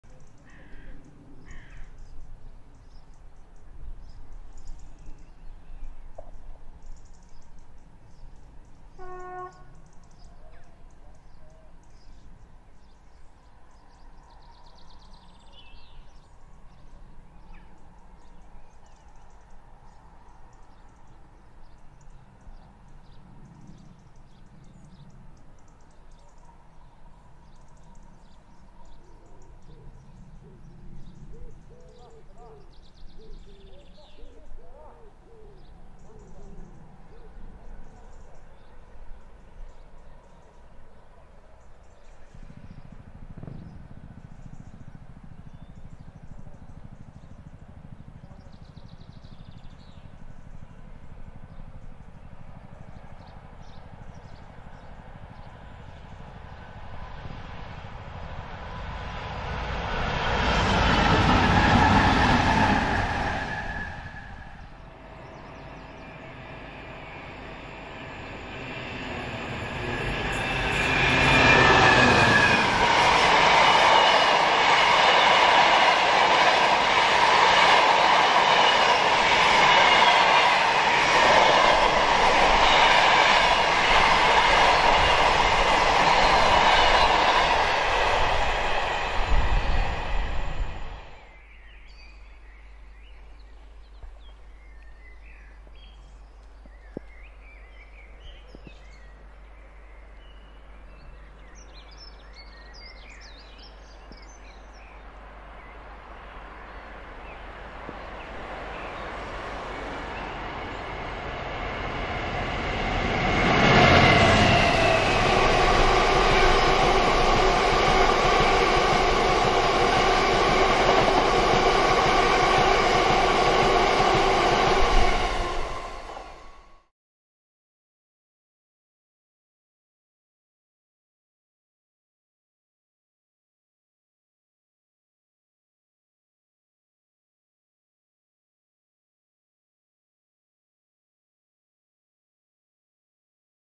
A pair of class 70's dragged by 66849 on the ECML
Seen descending Gamston Bank, Retford, the 0D01 Whitemoor Local Distribution Centre to Doncaster Carriage Holding Sidings formed with Colas 66849 'Wylam Dilly' hauling 70802 and 70803.

DBC 66065 speeding through Retford station with the 4E26 Dollands Moor Sidings to Scunthorpe Redbourne Siding running 55 minutes early.

DBC 66177 hauls the 6Z96 Wembley European Freight Operations Centre to Doncaster Belmont Down Yard running 46 minutes early just north of  Retford station.

Camera used - Panasonic HC-VX1.